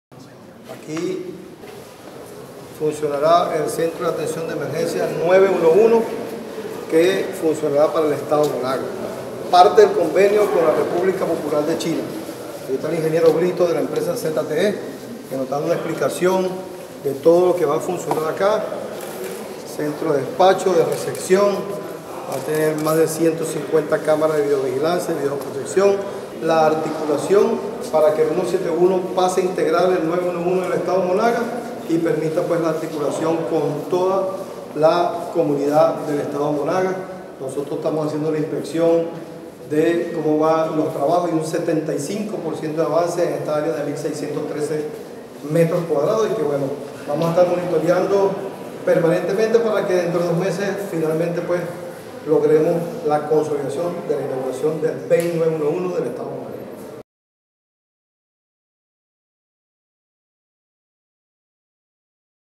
6Oct | @VEN911Oficial del Edo. Monagas
DatoImportante | Más de 150 camaras seran monitoreadas en esta nueva sede del @VEN911Oficial del Edo. Monagas en los proximos 2 meses.